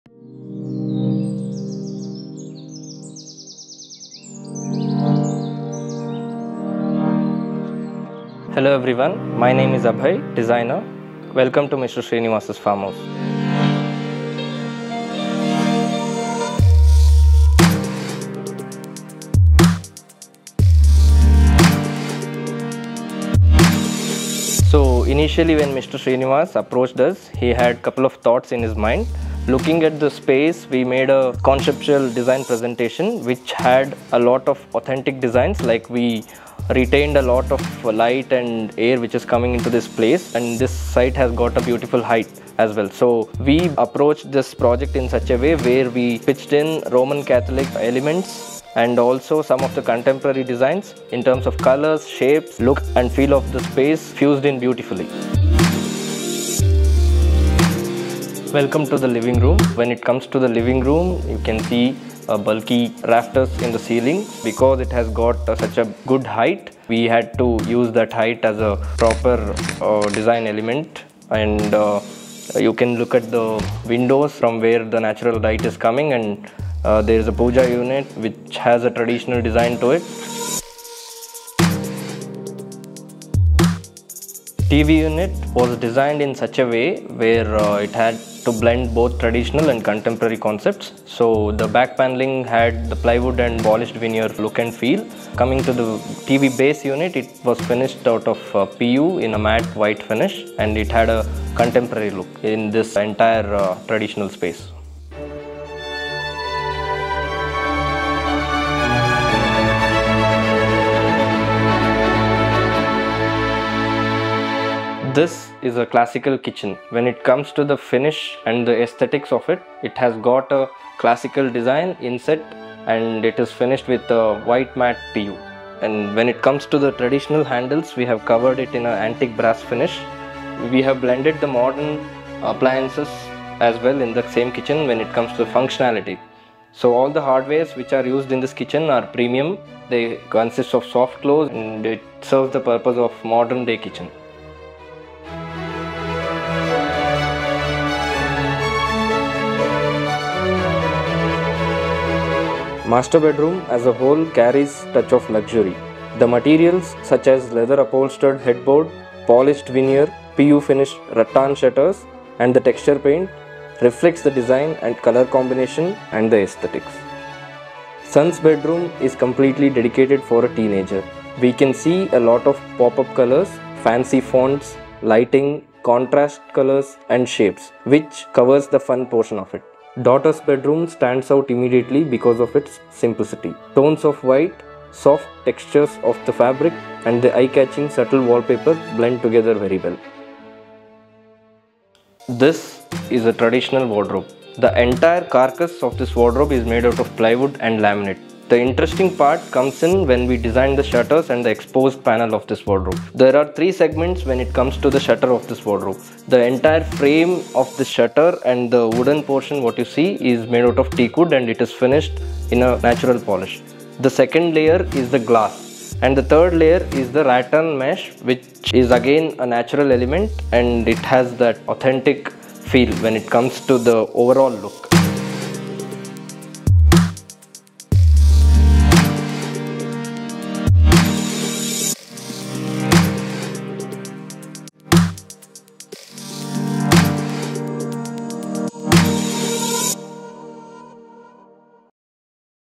Farmhouse Interiors by DreamSync | Luxury Interior Design | Home Tour
Welcome to an exclusive tour of Mr. Srinivas's breathtaking luxury farmhouse nestled in the outskirts of Bengaluru! Prepare to be captivated as we explore the seamless fusion of Roman Catholic and contemporary design elements that define this stunning residence. In this meticulously crafted home, every corner tells a story of elegance and sophistication. From the grand entrance to the cozy living spaces, every detail has been carefully curated to evoke a sense of timeless charm and modern comfort. Join us as we wander through the spacious rooms, adorned with exquisite furnishings and tasteful decor, showcasing the perfect balance between opulence and functionality. Whether it's the luxurious bedrooms, the inviting lounge areas, or the state-of-the-art kitchen, each space reflects the owner's impeccable taste and commitment to quality living. But it's not just the interiors that will leave you in awe. Step outside and be greeted by sprawling landscapes and lush greenery of the beautiful mango farm, creating an oasis of tranquility amidst the bustling city life. So come along and experience the epitome of luxury living as we take you on a journey through Mr. Srinivas's magnificent farmhouse, where tradition meets contemporary elegance in perfect harmony.